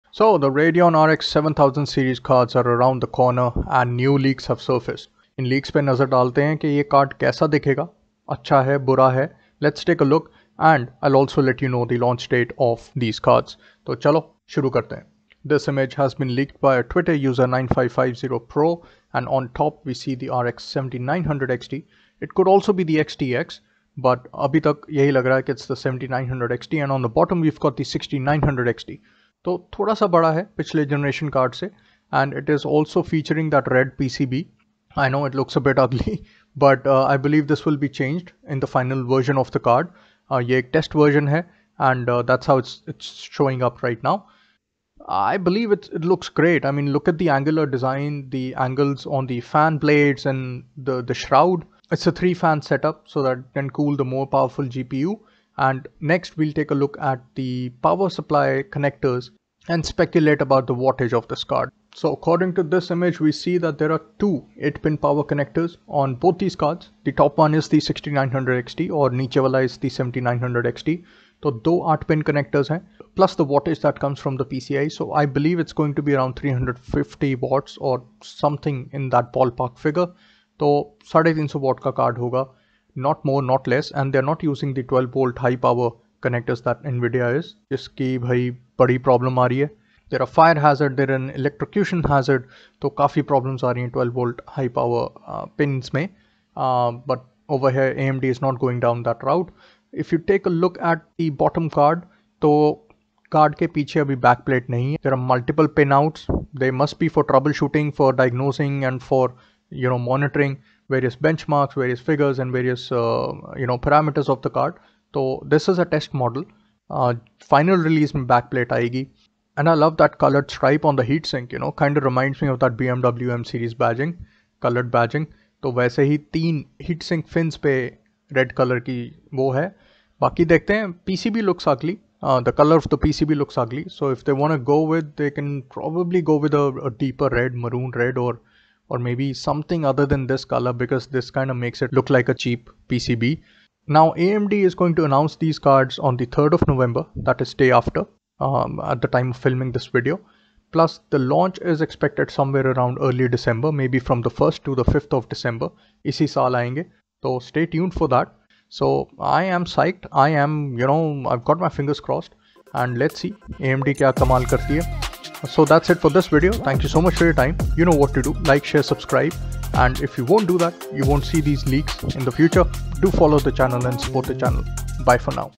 Surfaces and Announcement
New images have been leaked on the internet of the latest GPU line up from AMD, the AMD Radeon RX 7000 series which will be a direct competitor of the NVIDIA RTX 40 Series cards.
Check out the video for details on how the card looks like, the announcement date and the launch date..

- chipsets N frames